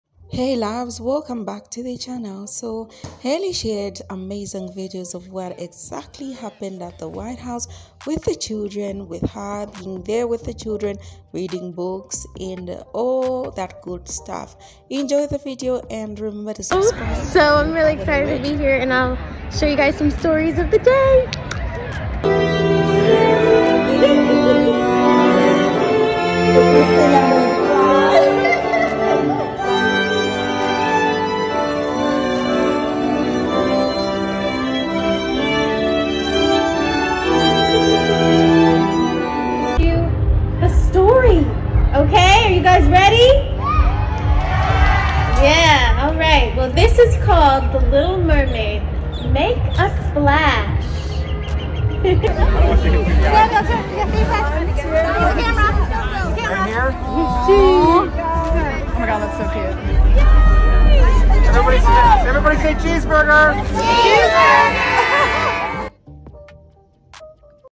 halle bailey at the white house with children(she is so sweet)
halle bailey shares shocking footage at the white house with children